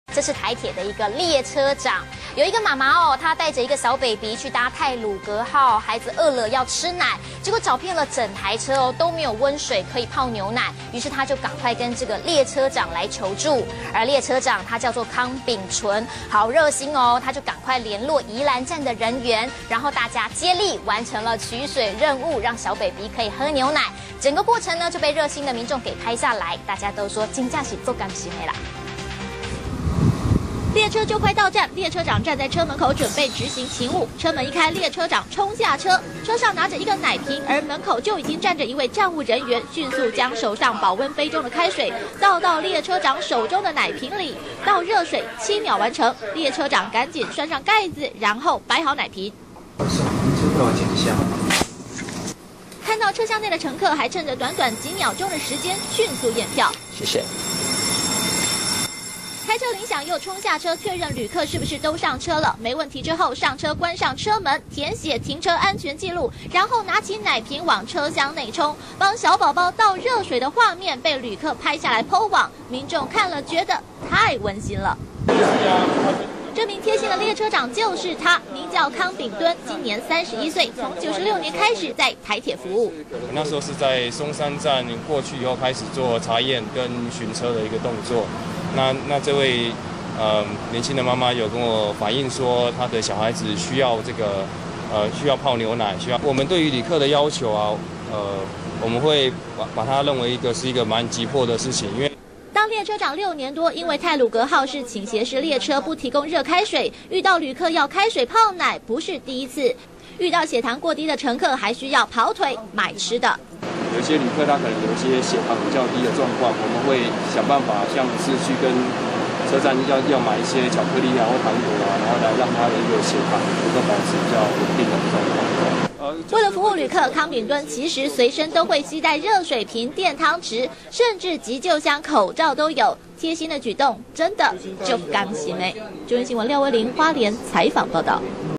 中天新聞》送暖！無溫水泡奶 列車長停站急取水
中天新聞》台鐵列車長真的足感心！有一名帶嬰兒搭乘太魯閣號的媽媽，因為小孩餓了要喝奶，找遍全車都沒有溫水可以泡奶，向台鐵列車長求助，列車長康秉惇聯絡宜蘭站人員接力完成取水任務，全程被熱心民眾拍下，民眾大讚真的好溫馨。